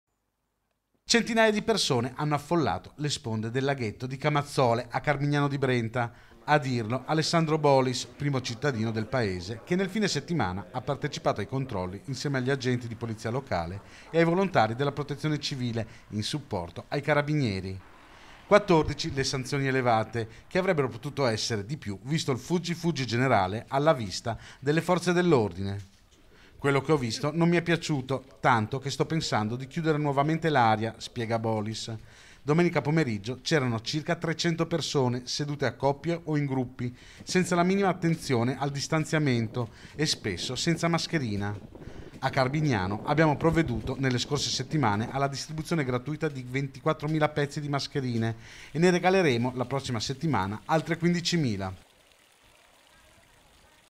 CONTROLLI SU CENTINAIA DI PERSONE LUNGO LE RIVE DEL LAGHETTO | 12/05/2020
TG Padova Sera 12/05/2020 - CARMIGNANO DI BRENTA - Weekend di controlli anche nell'alta padovana a cui il sindaco di Carmignano ha partecipato personalmente... - Servizio Redazione Medianordest, immagini Redazione Medianordest, montaggio Federico Cibotto - Segui Telenordest anche sul digitale terrestre! Visita il sito